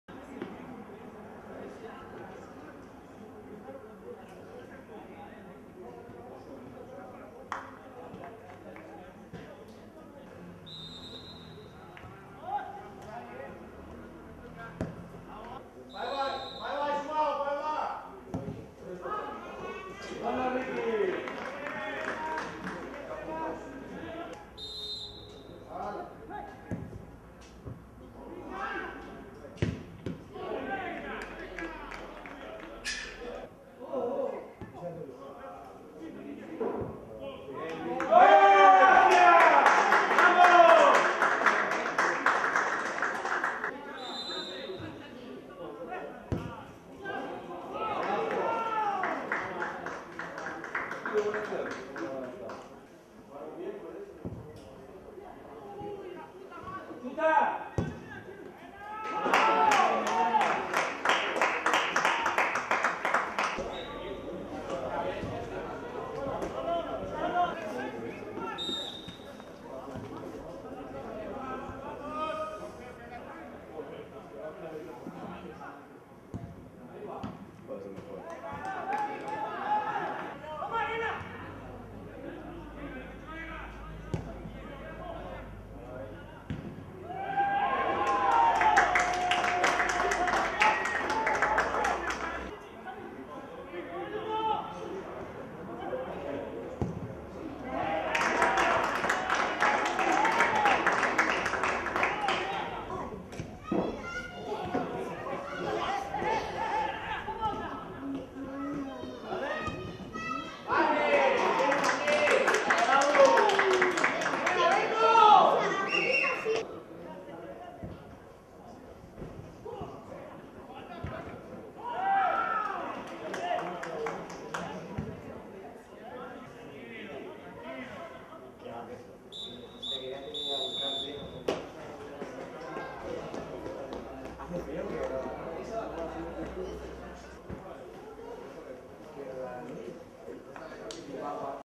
Inter Club Escaldes vs FC Santa Coloma (Lliga Nacional 11/12 - fase regular)
Lliga Nacional de Futbol 2011/12 (fase regular) - 1a Divisió - Jornada 8
Inter Club Escaldes vs FC Santa Coloma (0-7)
27/11/11 Camp d'Esports d'Aixovall